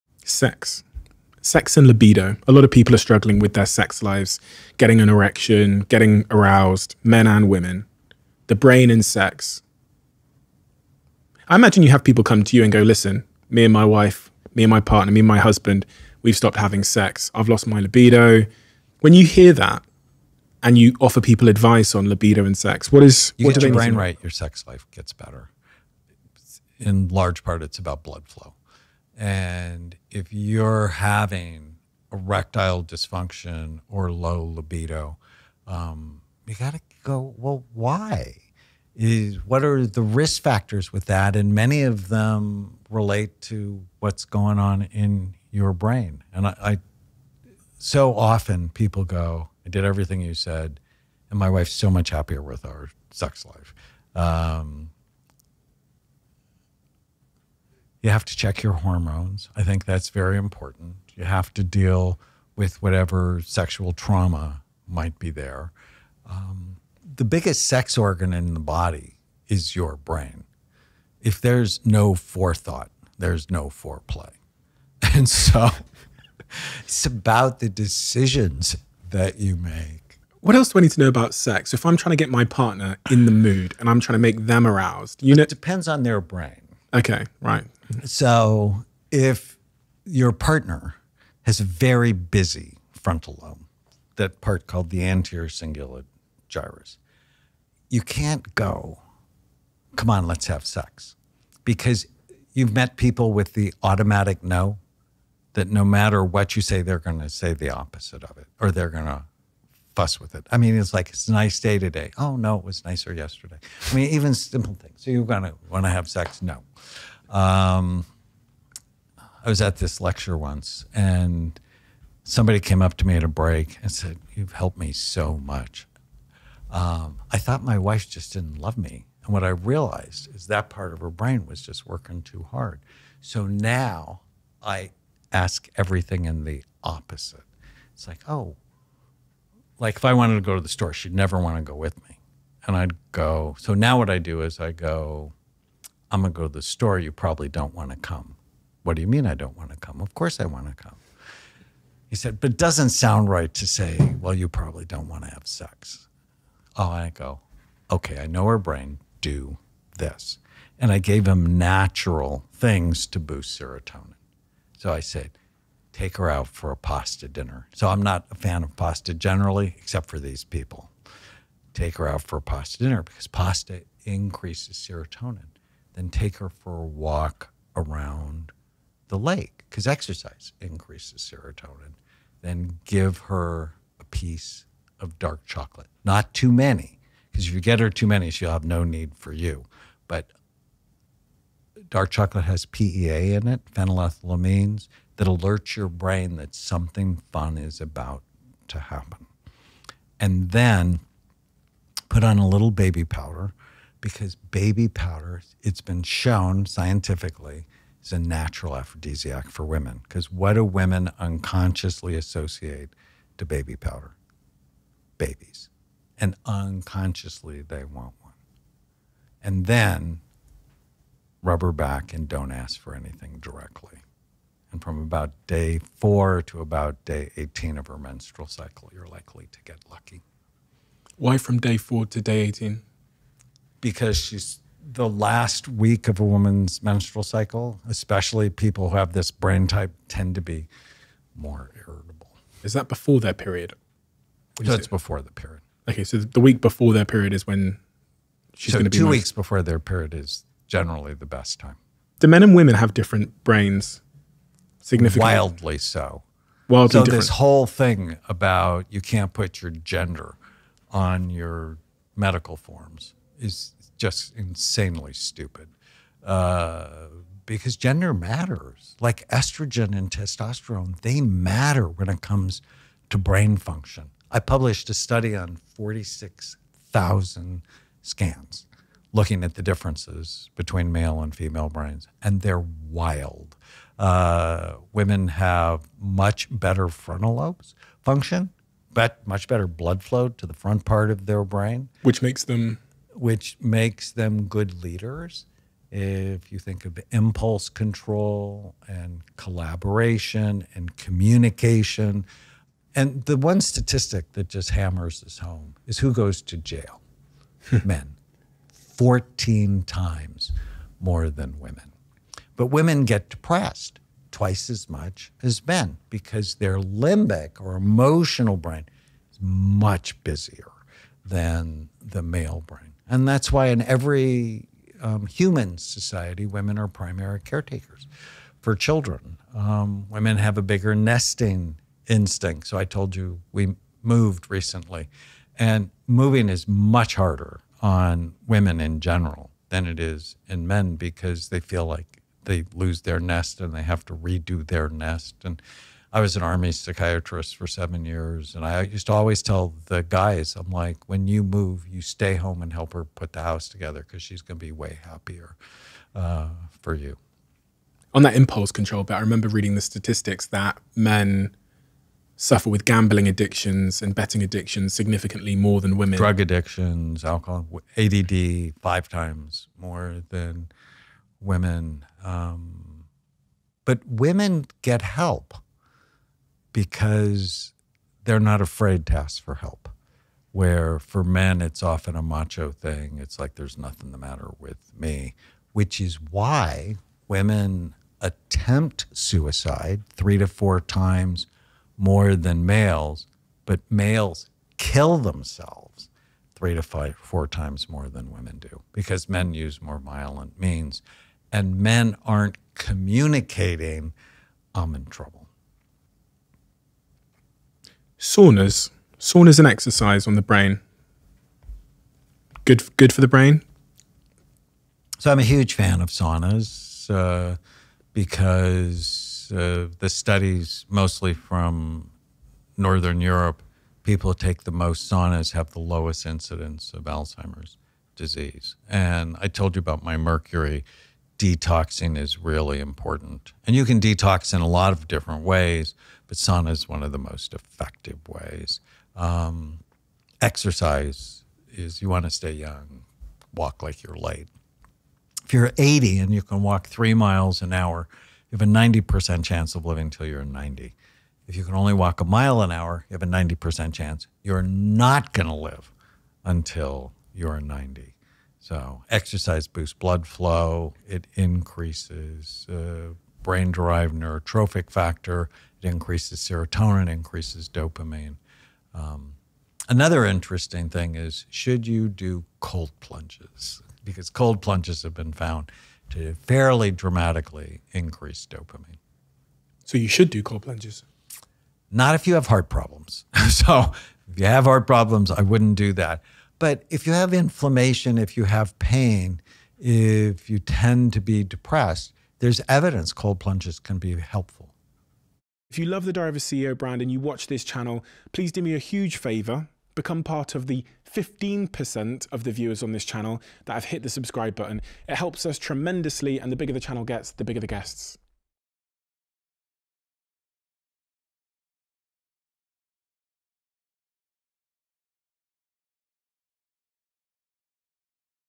The BEST Scientific Sex Advice All Men NEED To Know! | Dr Daniel Amen
Psychiatrist Dr. Daniel Amen reveals his best tips to hack the brain to increase libido and boost serotonin for yourself and your partner.